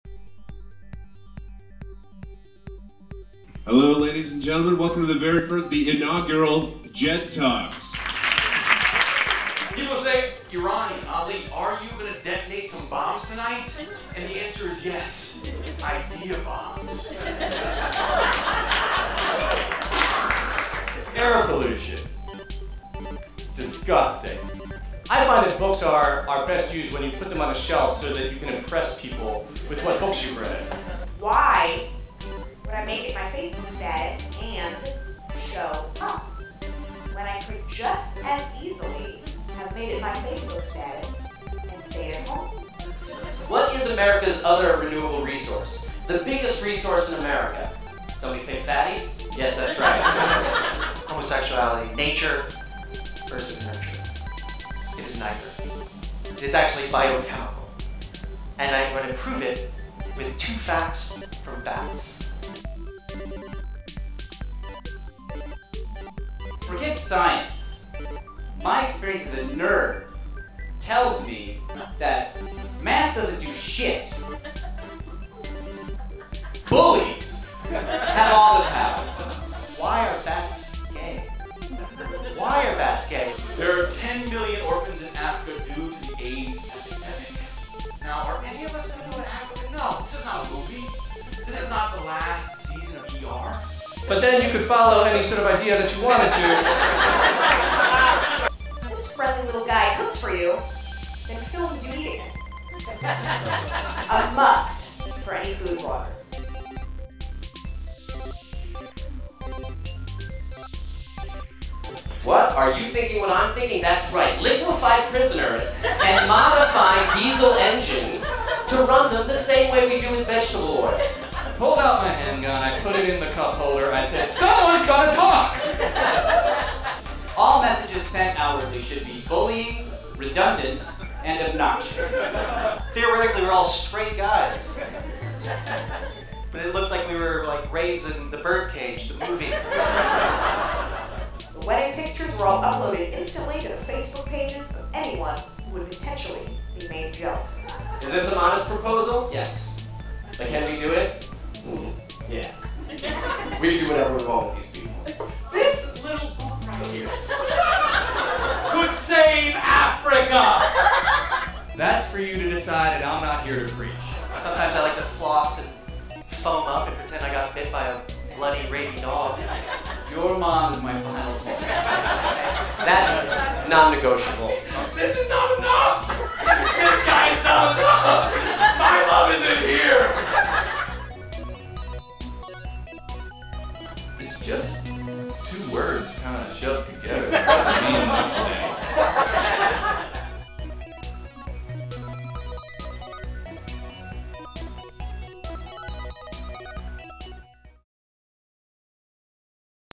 JED Talks 10/10 - Trailer
The trailer for the upcoming first batch of JED talks! Featuring talks by Kurt Braunohler, Erik Tanouye, CeCe Lederer, Will Hines, Bob Powers, Dan Allen and Ali Farahnakian. Hosted by Jed Concepcion.